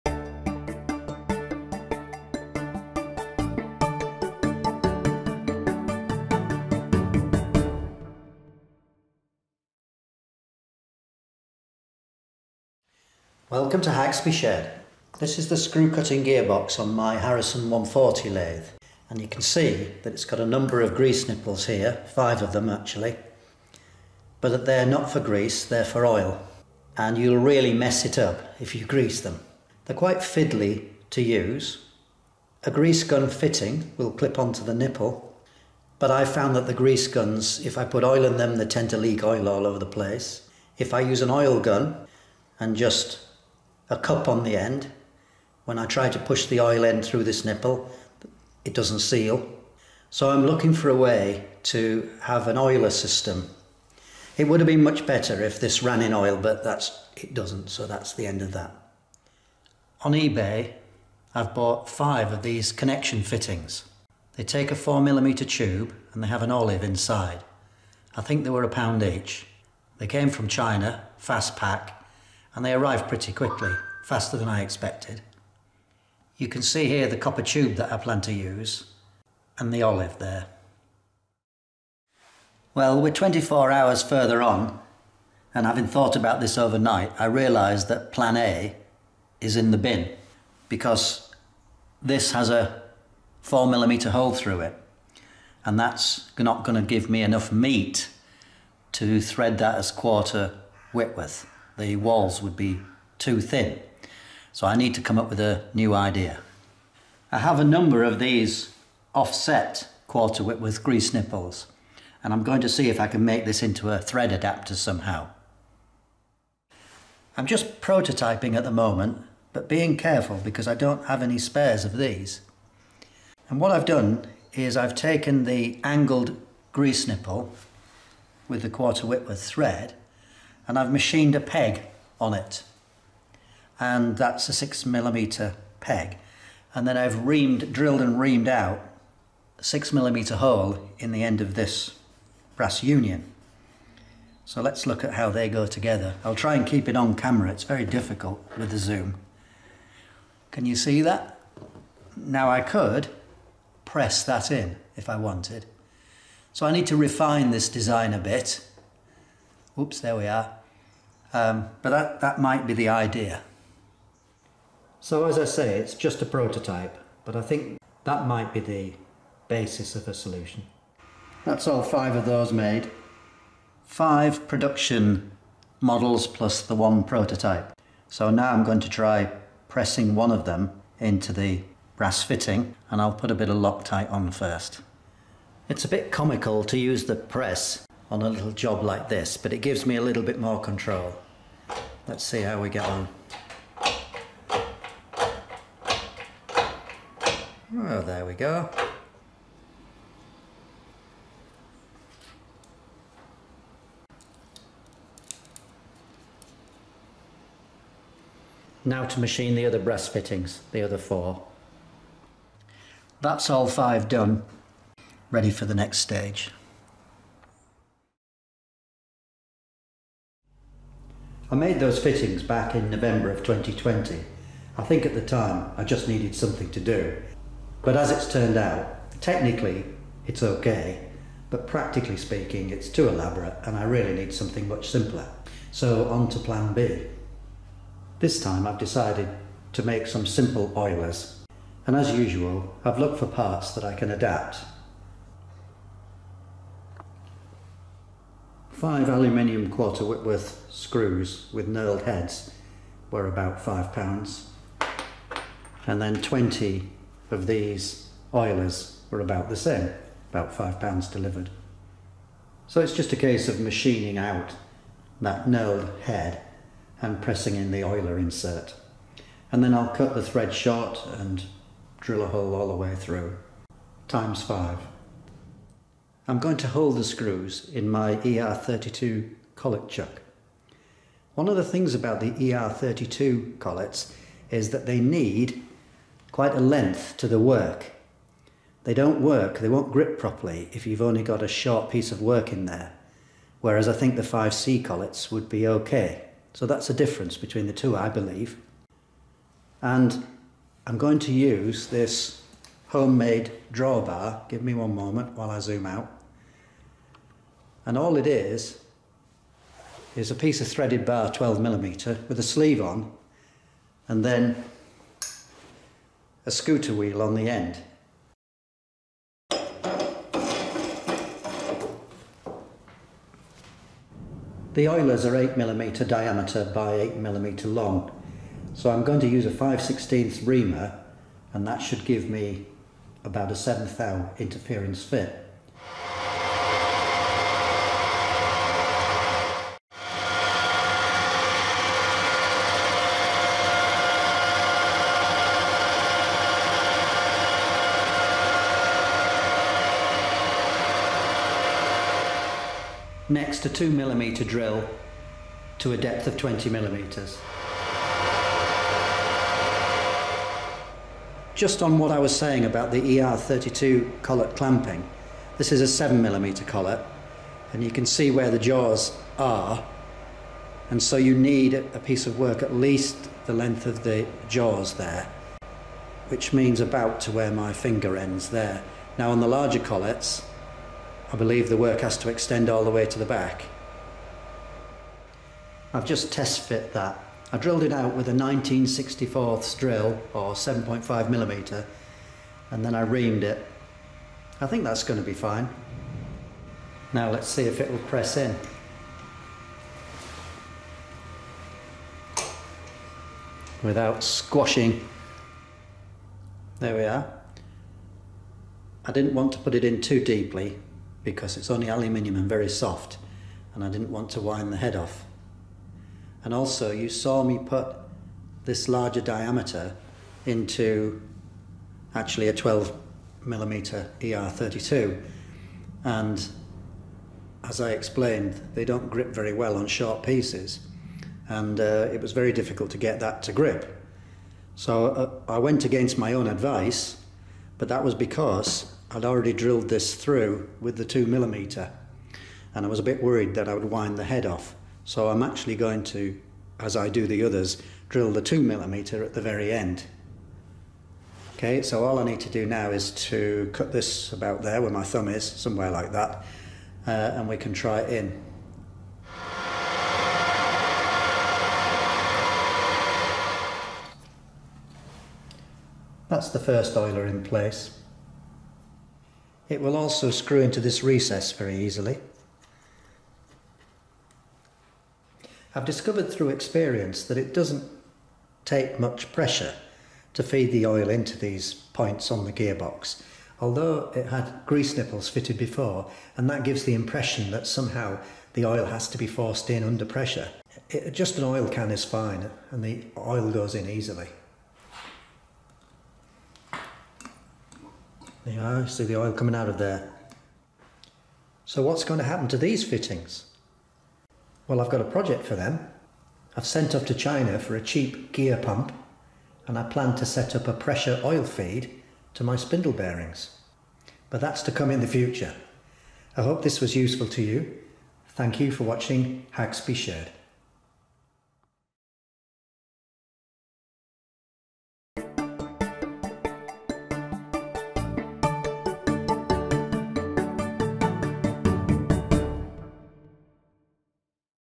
HS51 Screwcutting gearbox oilers for my Harrison 140 lathe
I made a set of oilers for my screwcutting gearbox, to replace the original grease nipples which were difficult to use.